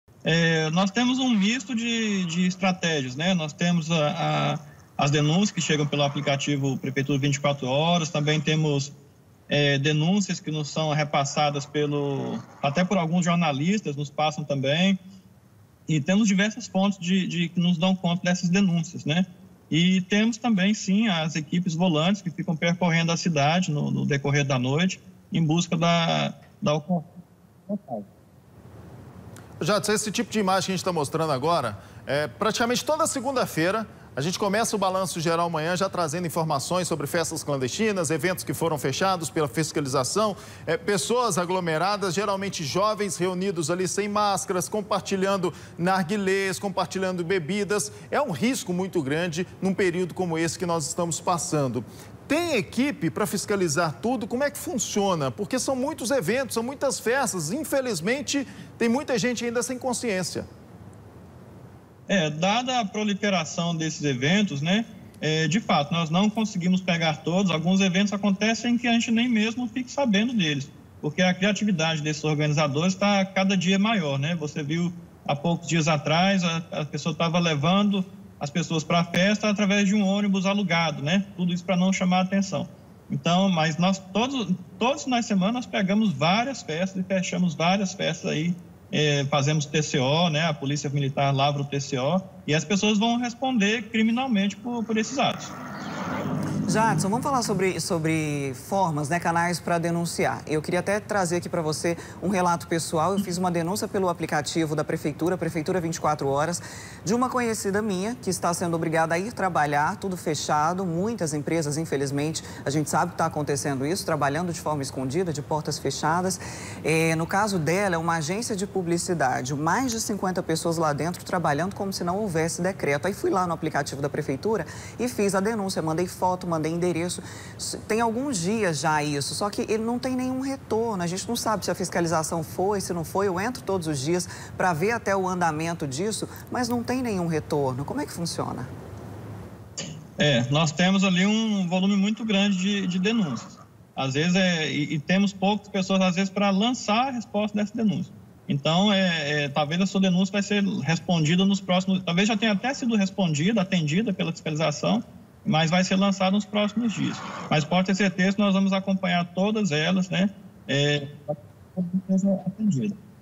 SEXTOU EM CASA: FISCALIZAÇÃO DE FESTAS CLANDESTINAS CONTINUAM
CHEGOU O FIM DE SEMANA E COM ELE VEM A IRRESPONSABILIDADE. A GENTE SABE QUE TODA SEMANA A HISTÓRIA SE REPETE. FESTAS CLANDESTINAS POR TODA CAPITAL. A GENTE FALA AGORA COM JADISON TAVARES DIRETOR DE VIGILÂNCIA SANITÁRIA E COORDENADOR DE FISCALIZAÇÃO.